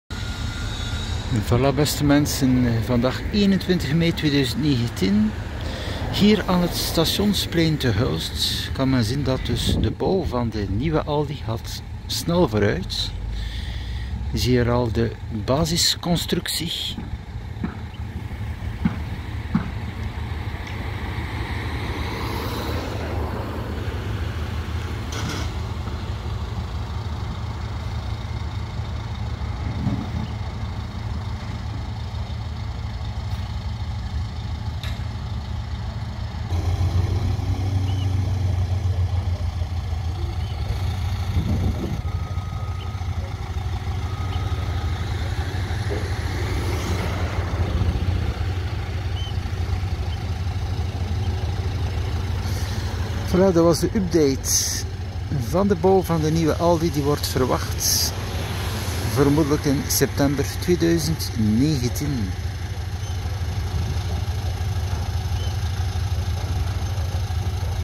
New aldi supermarket Hulst - update
The competition will become even tougher with this new, larger store that will be opposite the Albert Heijn supermarket. He moves from across the street to here. The aldi has only a few employees and the albert heijn 10 dozens of employees and this is also more family contact because you have human contact in every department, the aldi does not have this but the prices come under pressure because they are difficult times.